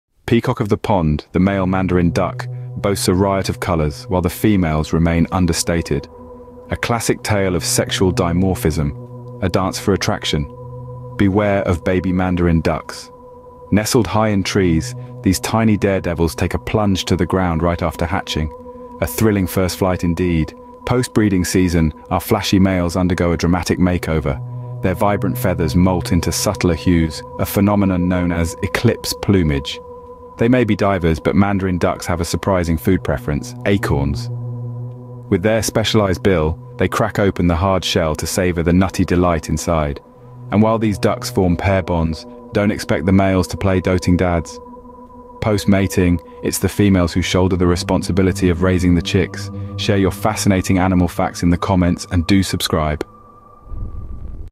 This Duck Looks Like a Painting! (Most Beautiful Bird in the World?)
Have you ever seen a duck that looks like it came straight out of a fairytale? The Mandarin Duck might just be the MOST STUNNING bird on the planet! From its vibrant COLORS to its CRAZY mating rituals, this duck will BLOW YOUR MIND! Get ready for a FEAST FOR THE EYES as we explore the incredible world of the Mandarin Duck!